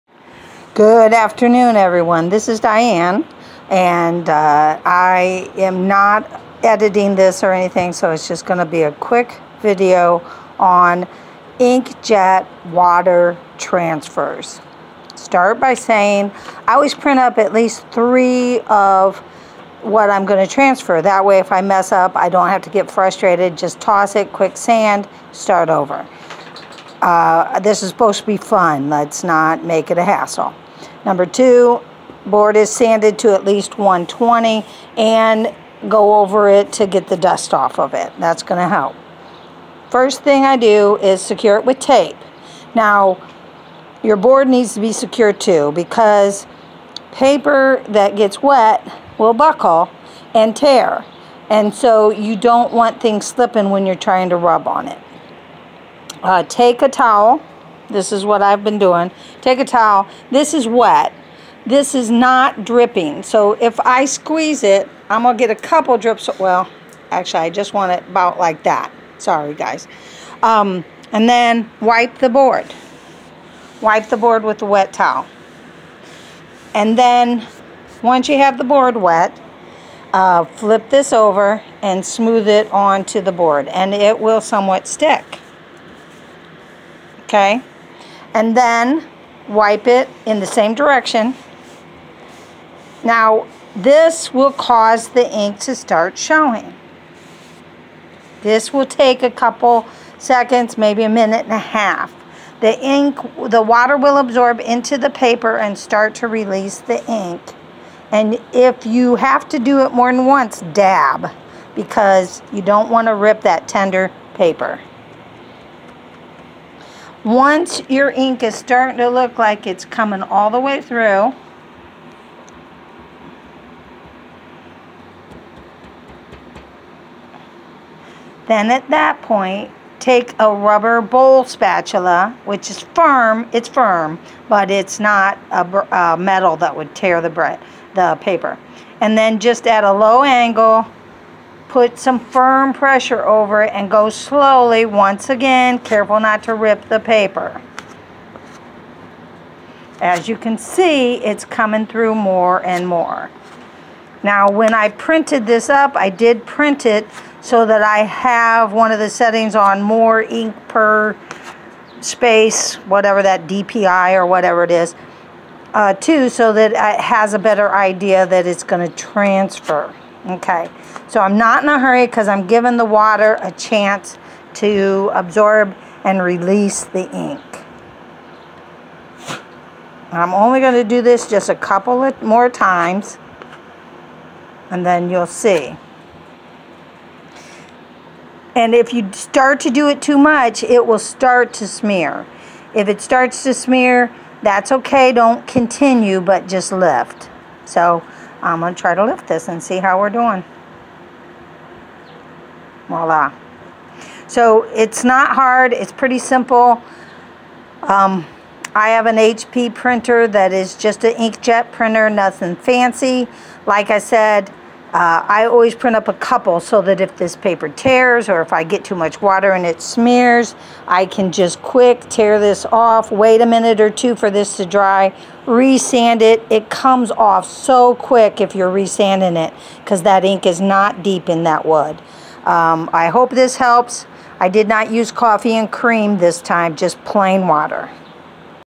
Inkjet image transfer to wood USING WATER
This is a simple video showing how to transfer a simple injet image to wood for use in carving or other craft projects.

Image transfer to wood Using an Inkjet Printer and Water.
1. Print image in mirror.
2. Sand your wood to at least, the paper has to connect with the wood so any grooves will not catch the ink.
3. Secure the image face down to the wood where you want it. I tape on one side so I can peak to see if it is working without moving it too much – but try not to move much.
4. Get a damp cloth.  It should feel wet, but not too wet.
5. Wipe the wood down with water.  This helps the paper stick and removes any lingering dust.
6. Smooth the paper down.  Be gentle.  This is where the ink will start to come off the paper. Take the wet rag and go over the paper.  It won’t look wet at first – it takes a couple of seconds for the paper to absorb. Be slow about this, it takes several seconds to absorb and look wet.  If you need to wet more than once – just dab so that you don’t tear wet paper.
7. I use the flat side of a dollar store plastic bowl scraper.  It is firm but still plastic.  When applying pressure use a low angle and firm pressure.  Remember – you do not want your paper to move or tear.
8. The ink will start to show on the back side.  As it is loosening up off the paper onto the wood it also becomes easier to see on the back side.  So if there are spots the do not show threw, dab a couple of times with the same moist rag.
9. Check to see if it transferred.